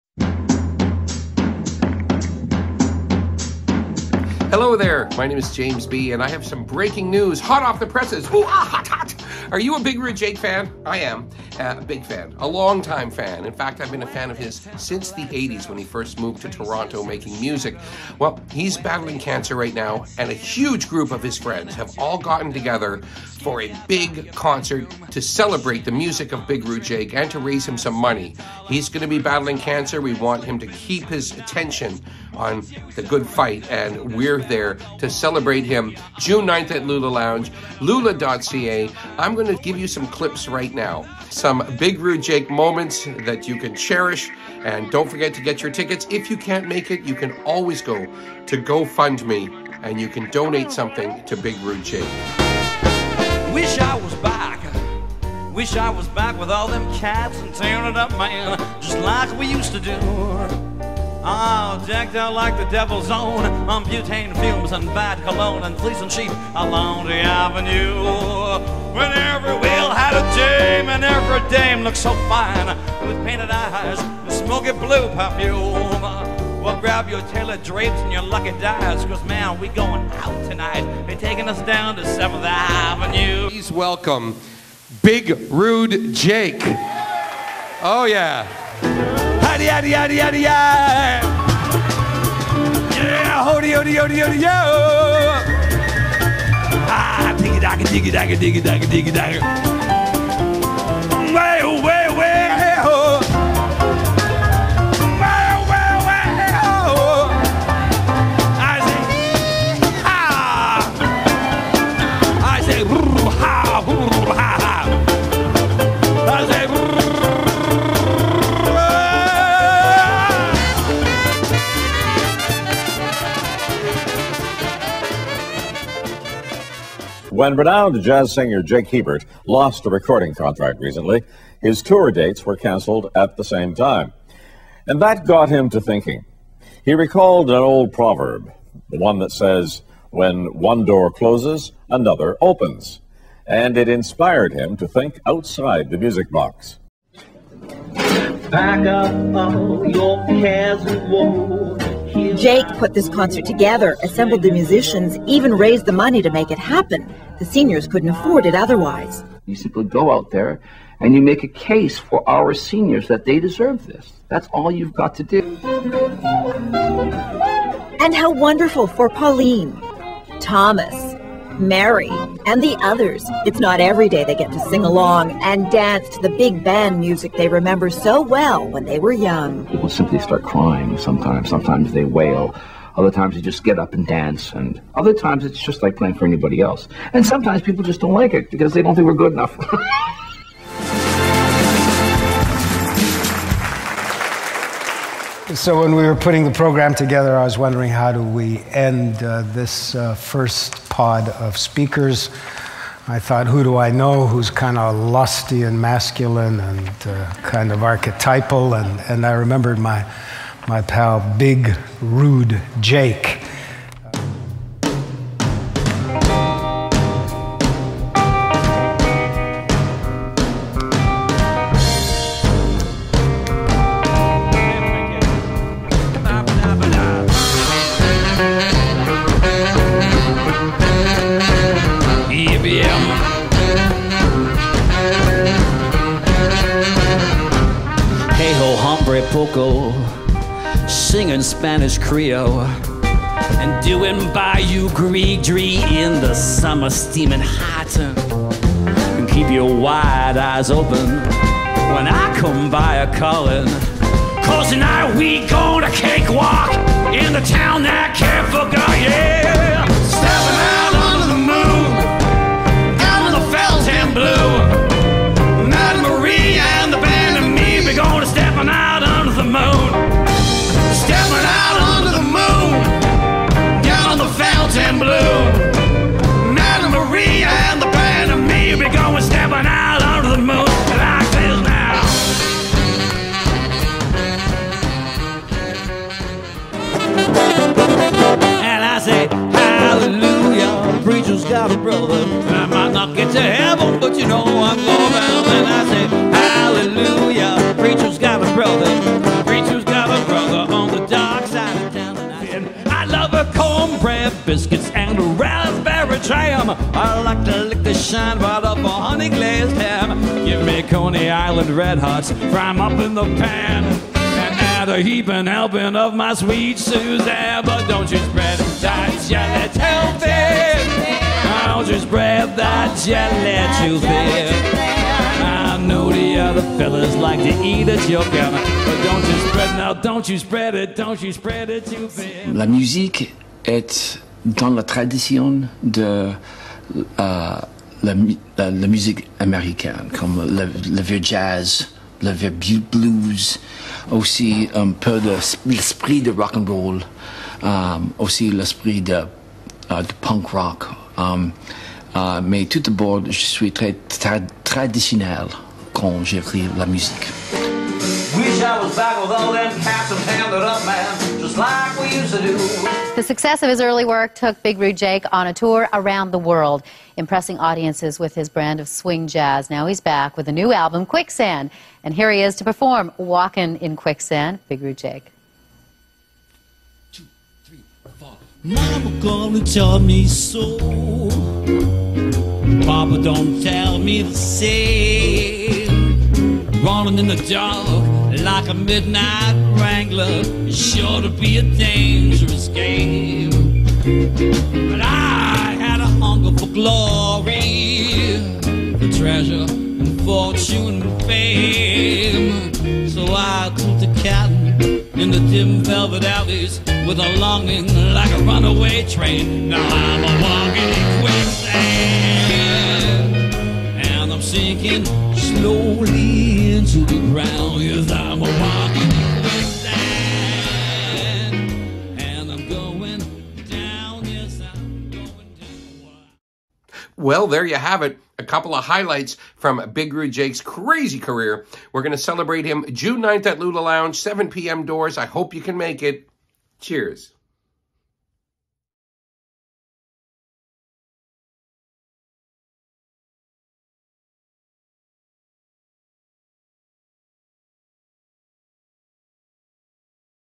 (Don't miss) *JAKEFEST* 9 June 2022!
For many years Big Rude Jake (Jake Hiebert) has given his time and incredible talents to so many worthy causes, but now he's the one who needs a bit of help.
Jake is in a no holds barred battle against Cancer. In order for him to focus on the fight, he needs some help to make sure that his family has a financial buffer while he is unable to support them.
JAKEFEST has been organized by Jake's friends and fellow musicians to raise some much-needed funds to ease some of the burdens of the Hiebert family (Anna-Lisa his wife and his 7-year-old daughter Hope).
Come on out to a star-studded revue with some of the best musical friends of Jake on June 9th at Lula Lounge in Toronto.
Tickets are $20 in advance (see link) or $30 at the door. Doors open at 7pm and the show starts at 8.

Tickets are general admission. Seating will be on a first-come, first-serve basis; standing room after seats are filled.
The show ticket does not include food or beverages.
Attending for dinner at Lula's do not include a show ticket.
If you can't make the show, but would still like to help Jake and his family, please consider a donation to the Go Fund Me initiative that has been set up.